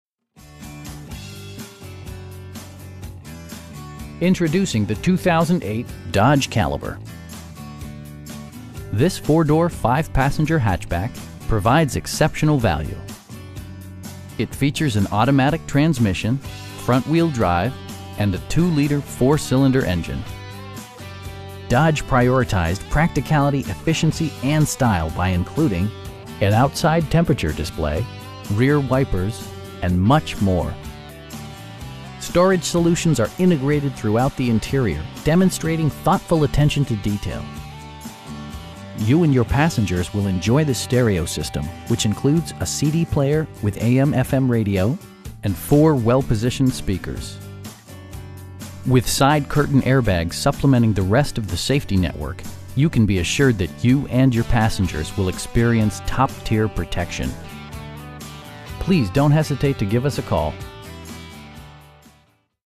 2008 Dodge Caliber SE in Moline, IL 61265
Sexton Ford
3802 16th Street

Introducing the 2008 Dodge Caliber. This 4 door, 5 passenger hatchback provides exceptional value! It features an automatic transmission, front-wheel drive, and a 2 liter 4 cylinder engine. Dodge prioritized practicality, efficiency, and style by including: an outside temperature display, front bucket seats, tilt steering wheel, rear wipers, and much more. Storage solutions are integrated throughout the interior, demonstrating thoughtful attention to detail. You and your passengers will enjoy the stereo system, which includes a CD player with AM/FM radio, and 4 speakers, providing excellent sound throughout the cabin. With side curtain airbags supplementing the rest of the safety network, you can be assured that you and your passengers will experience top-tier protection. We pride ourselves in consistently exceeding our customer's expectations. Please don't hesitate to give us a call.